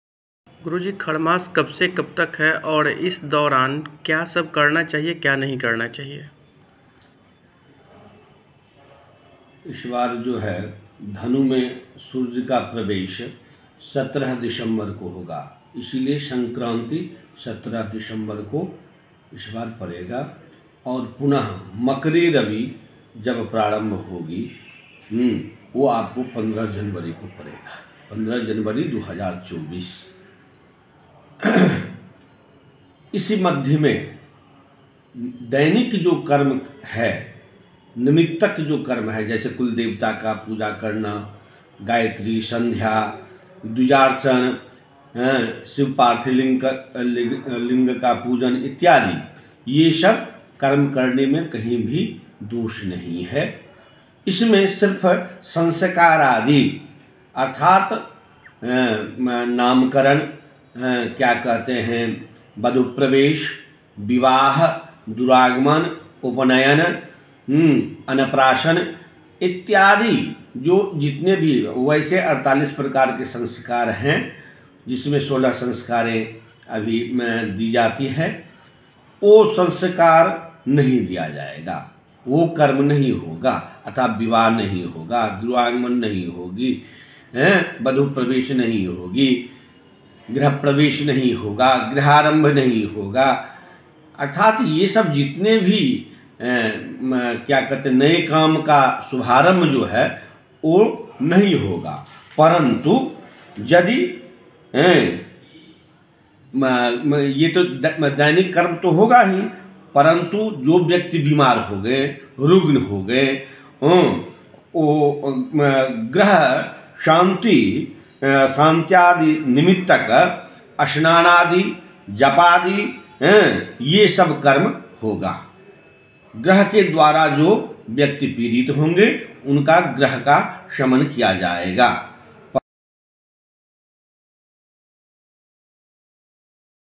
दिसंबर में इस दिन के बाद नहीं कर पाएंगे कोई भी शुभ कार्य | Kharmas 2023
There will be a break of 1 month on all kinds of auspicious work. According to the scriptures, no auspicious activities like marriage, upanayana, mundan, house warming, house opening, Duragamana, naming ceremony are done in Kharmas.
सभी तरह के मांगलिक कार्य पर पूरे 1 महीने का ब्रेक लग जाएगा . शास्त्रों के मुताबिक खरमास में किसी तरह के मांगलिक कार्य जैसे विवाह , उपनयन , मुंडन , गृह प्रवेश  , गृह आरंभ , दुरागमण  , नामकरण जैसी कार्य नहीं की जाती है .